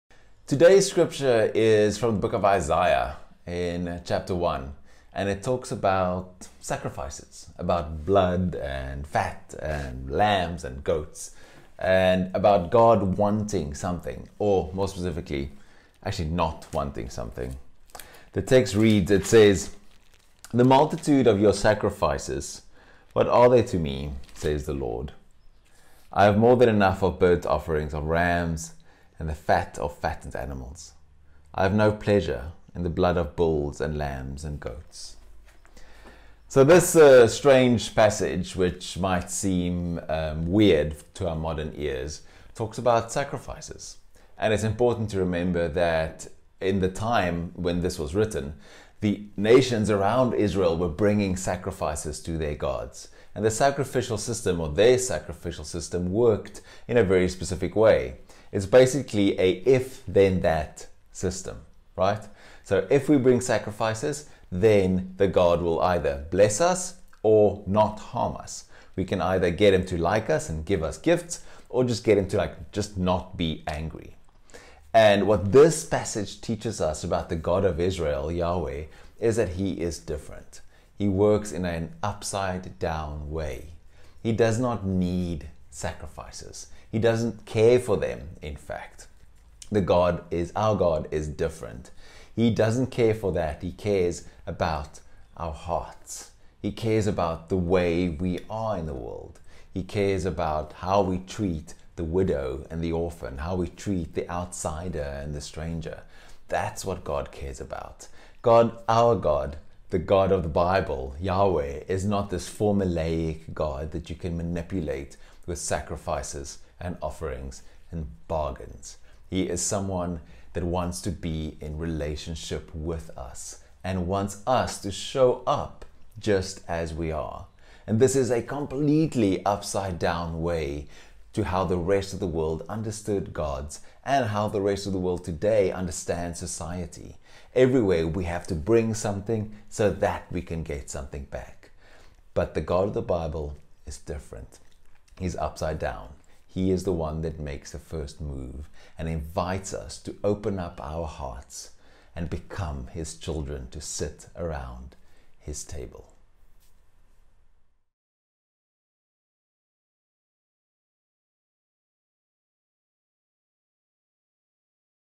Matins - Don't Bring Your Goat
This week's teaching is brought to you from Pierre Du Plessis, Pastor at InVia, Cape Town, South Africa. Today's teaching is drawn from Isaiah 1